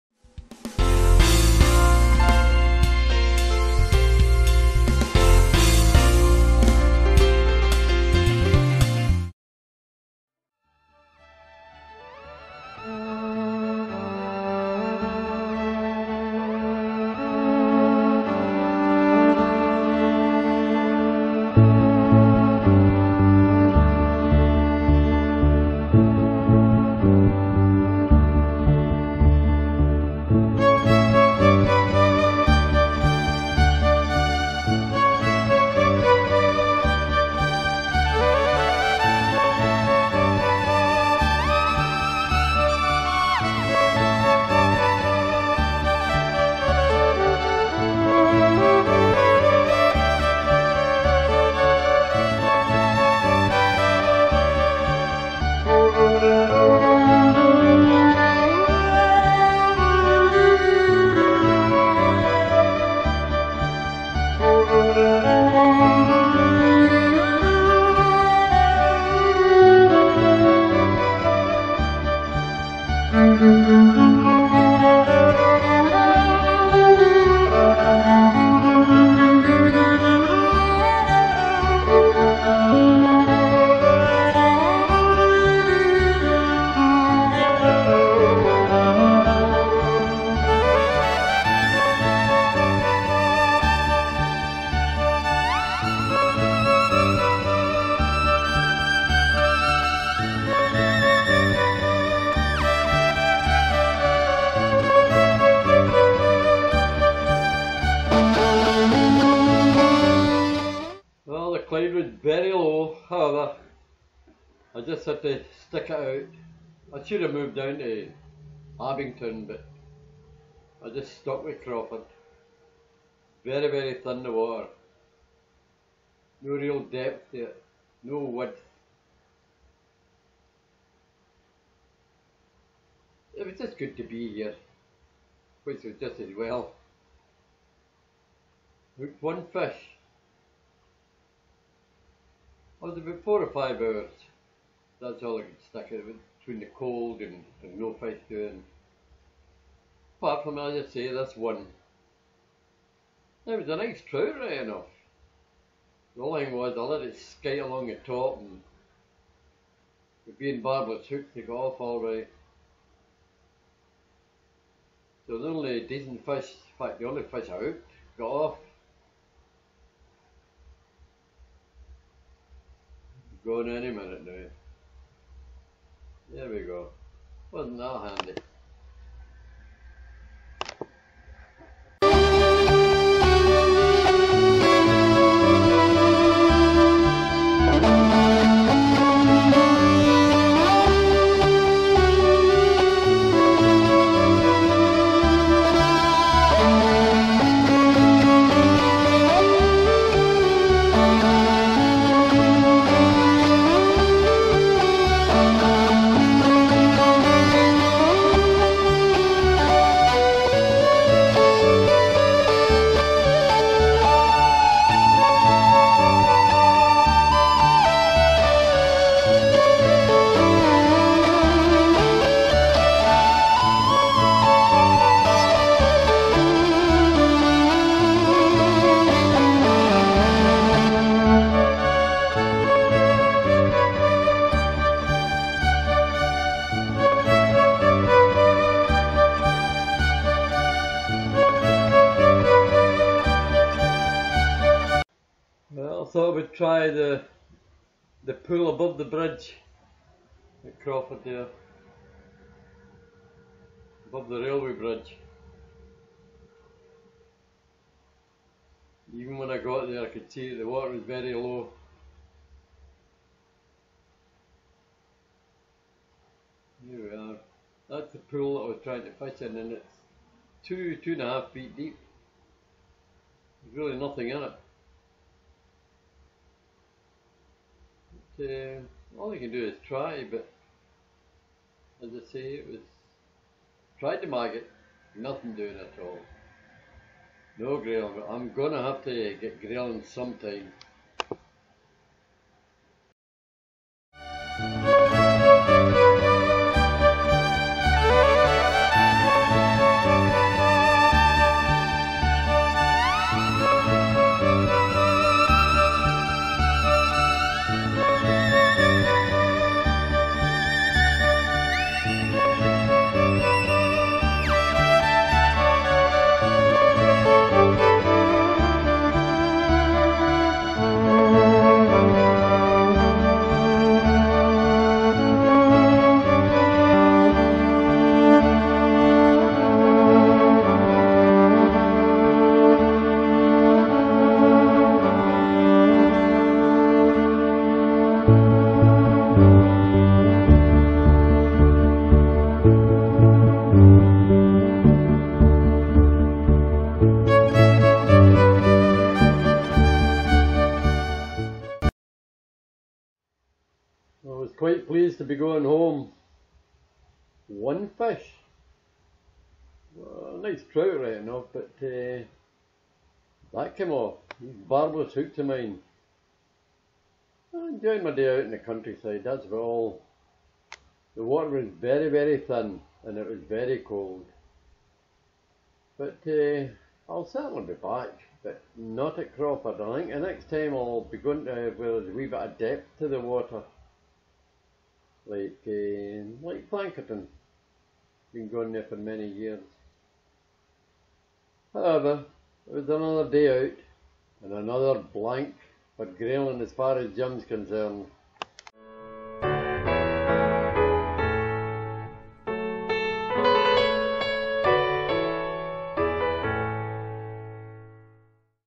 another blank grayling trip
hooked and lost one trout ( frustrating )